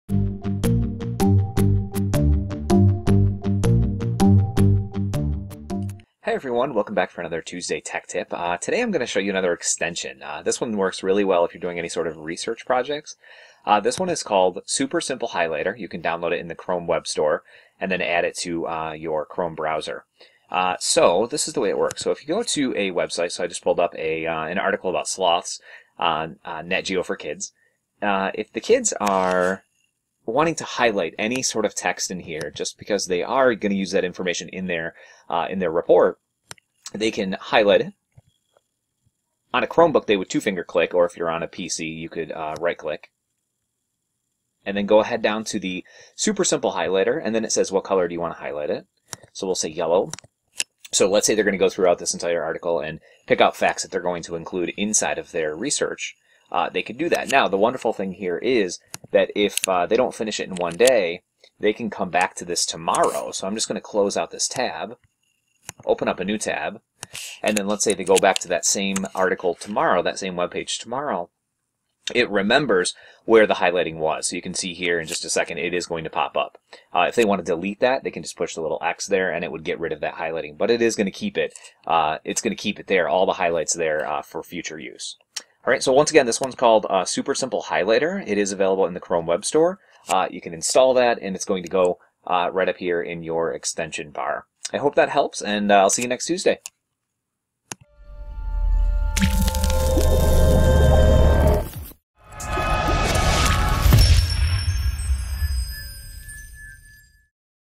Tuesday Tech Tip- Super Simple Highlighter
An easy way to use a highlighter in any online article.